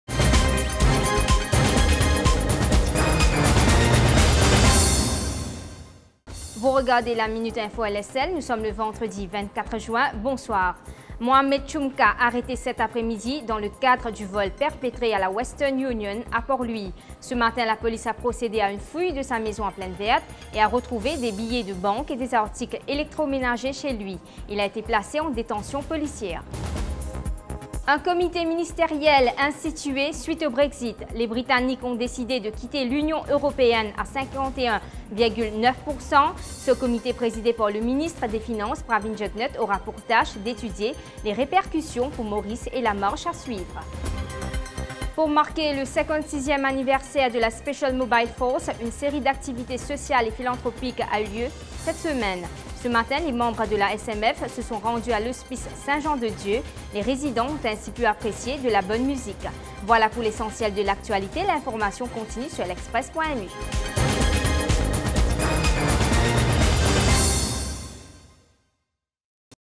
LaMinuteInfo: deuxième arrestation après le vol à Western Union
A la Une de La Minute Info LSL du vendredi 24 juin: deuxième arrestation après le vol commis à Western Union. Aussi dans l’actualité, un comité interministériel a été mis sur pied pour se pencher sur les conséquences de la sortie du Royaume Uni de l’Union européenne.